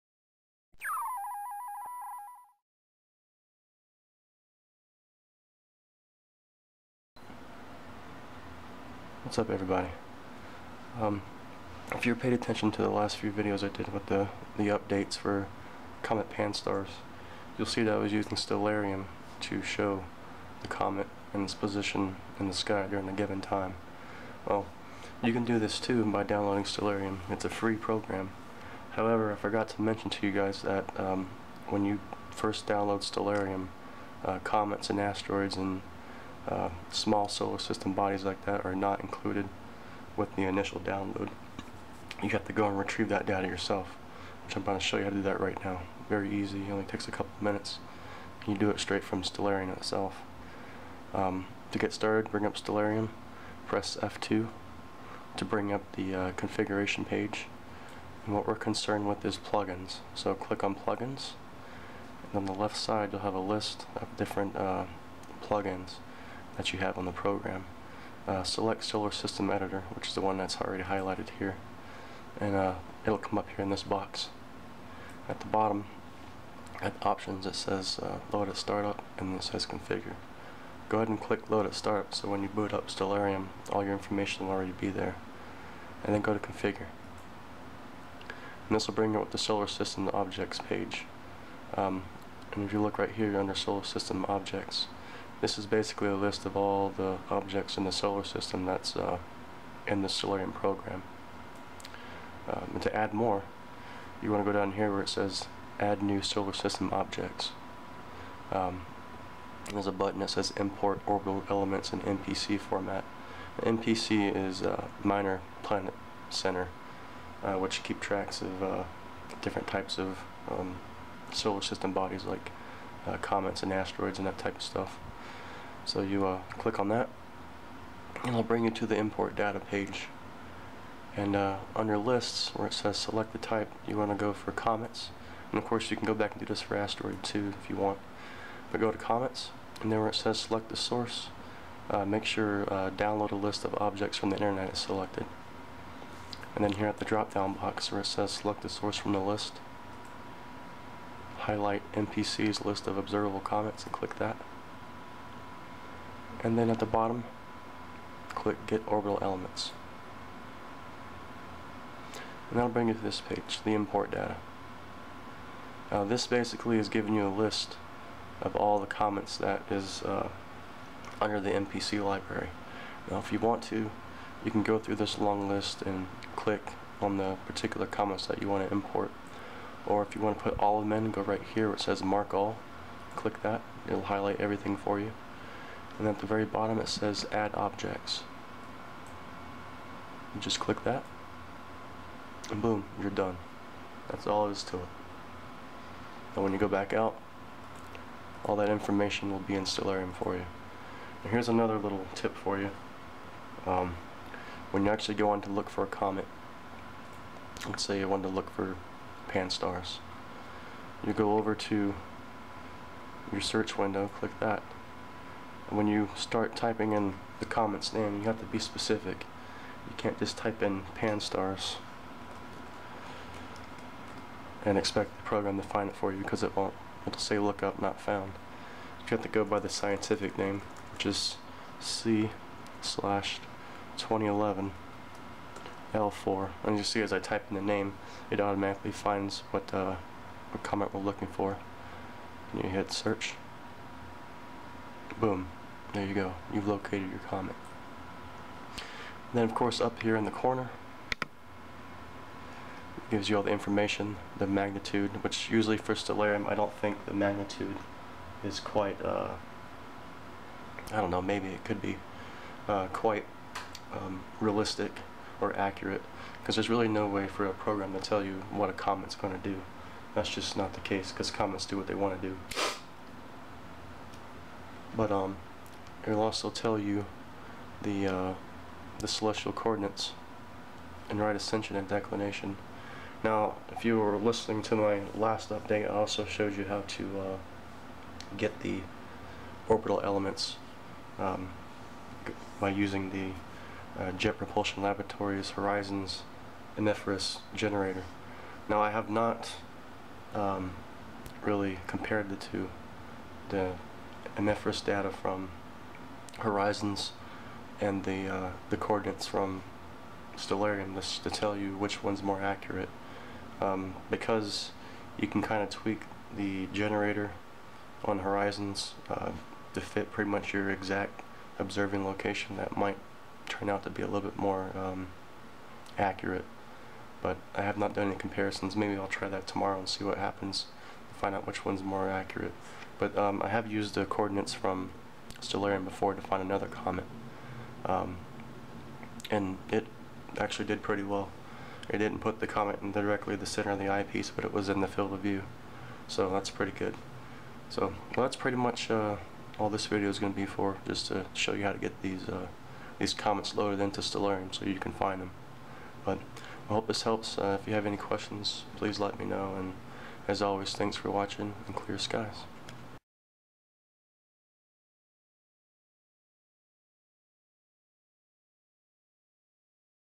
How to add comets to Stellarium
This is a video tutorial on how to import comets and orbital data to Stellarium.